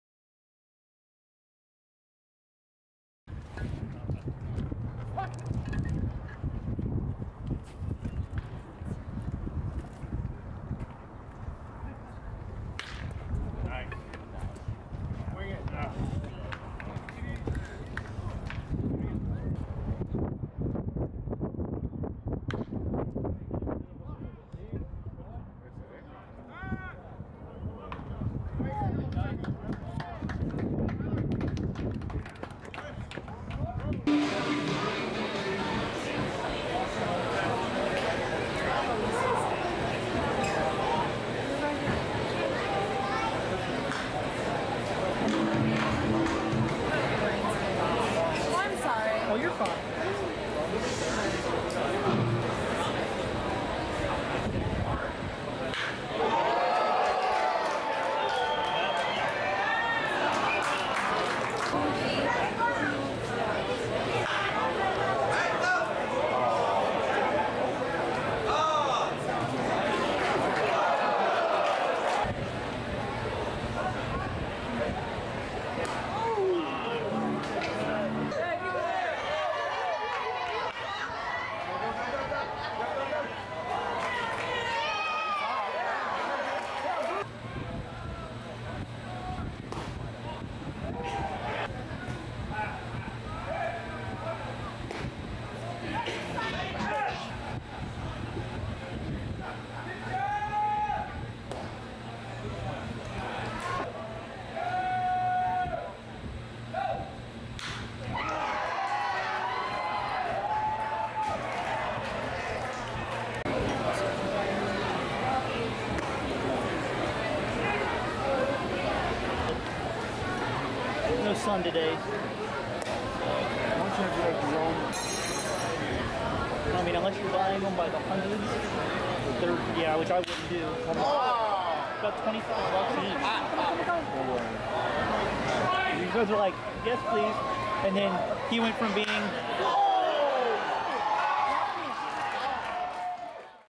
Tyler Stephenson hitting in 2018 for the Cincinnati Reds
Cincinnati Reds prospect Tyler Stephenson hitting during spring training and with the Daytona Tortugas during the 2018 season against the Fort Myers Miracle.